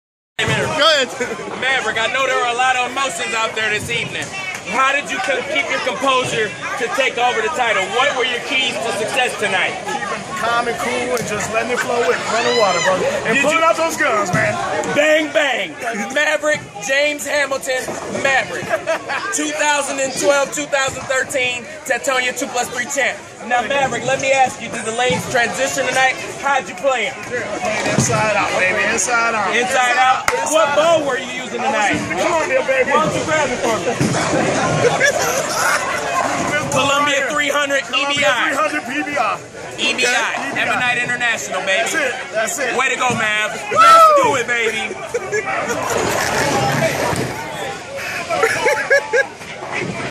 Courtney crape interviews James maverick Hamilton
Amf Bowlero